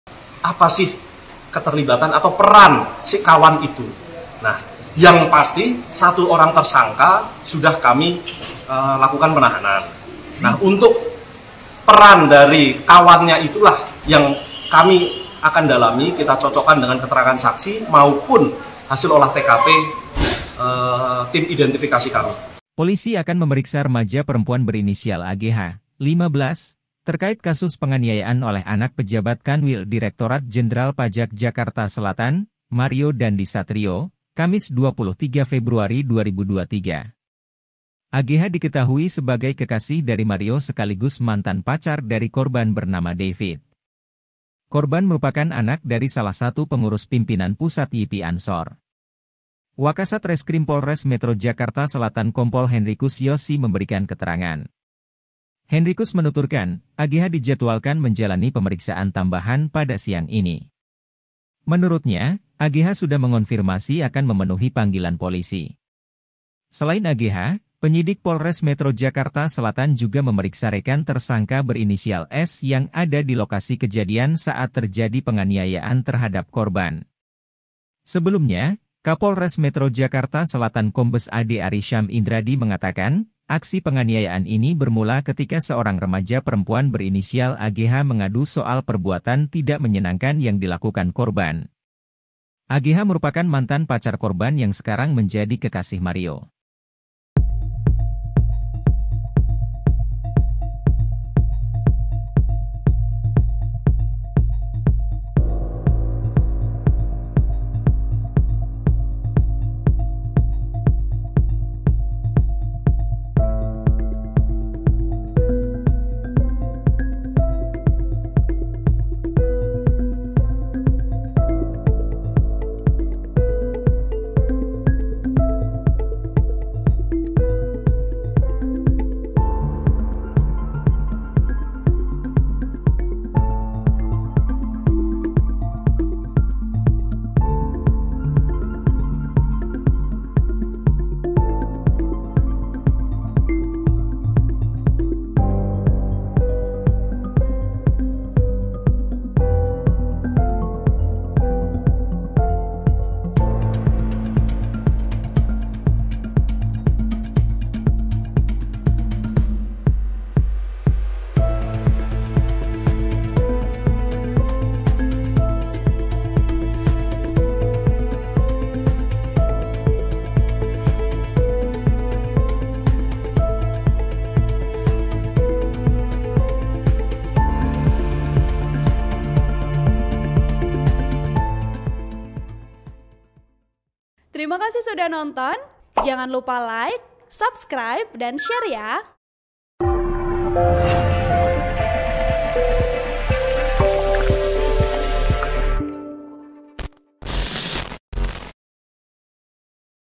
Kepolisian Turut Periksa Pacar Mario Anak Pejabat DJP, Diduga Terlibat dalam Penganiayaan David
TRIBUN-VIDEO.COM - Polisi periksa wanita inisial AGH (15), pacar Mario Dandy Satriyo (20), anak pejabat Kanwil Direktorat Jenderal Pajak Jakarta Selatan, Kamis (23/2/2023).

AGH ada di lokasi saat Mario menganiaya David, putra pengurus PP GP Ansor, sampai koma. Aduan AGH juga yang memicu Mario aniaya David.

Sebelum berhubungan dengan Mario, AGH pernah menjadi pacar David.